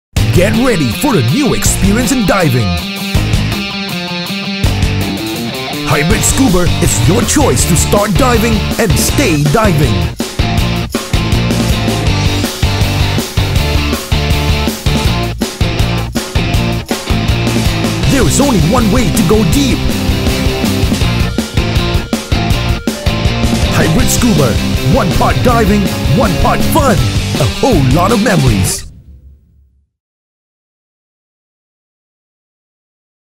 Hybrid Scuba Promotional Clip
Hybrid Scuba offers the best in diving for all levels of divers. Based in Singapore, Hybrid Scuba offers overseas dive trips to some of the top dive sites in South East Asia and beyond. for a brand new experience in diving!  Hybrid Scuba...One part diving, one part fun and a whole lot of memories!